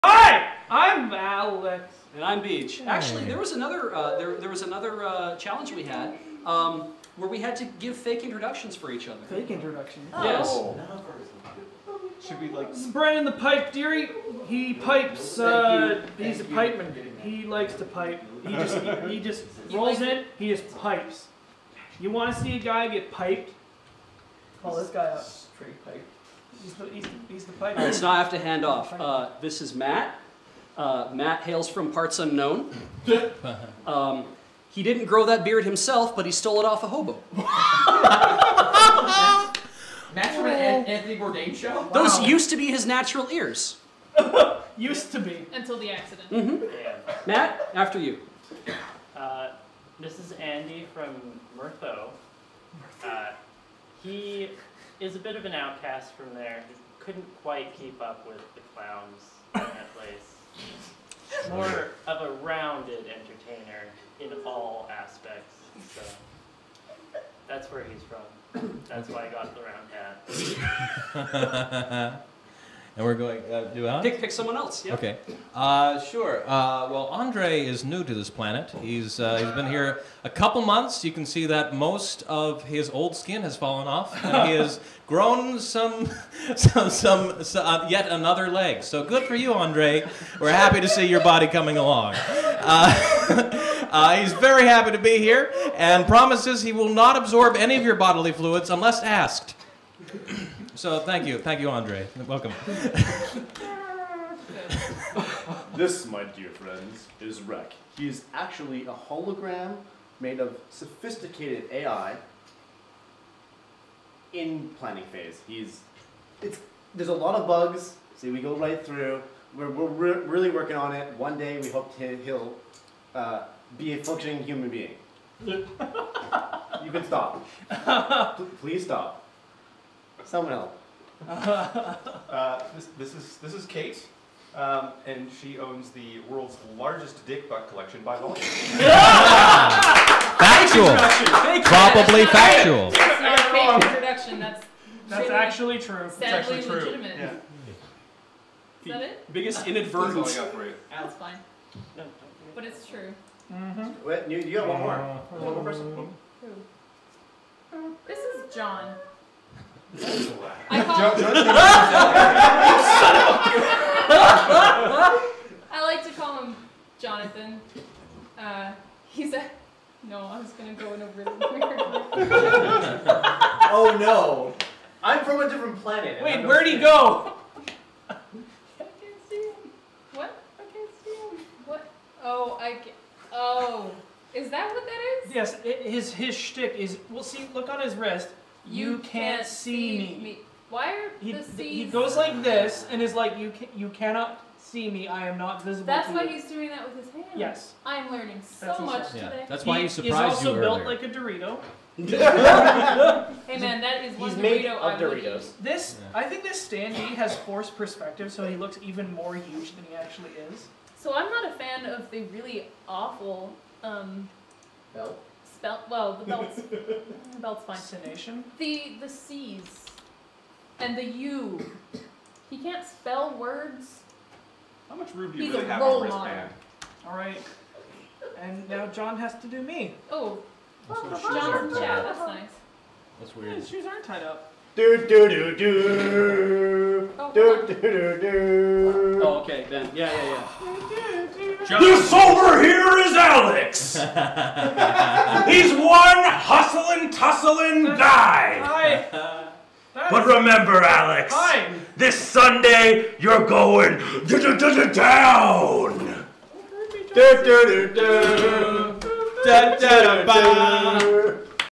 DB9 - Everyone gives fake introductory speeches about each other
Everyone gives fake introductory speeches about each other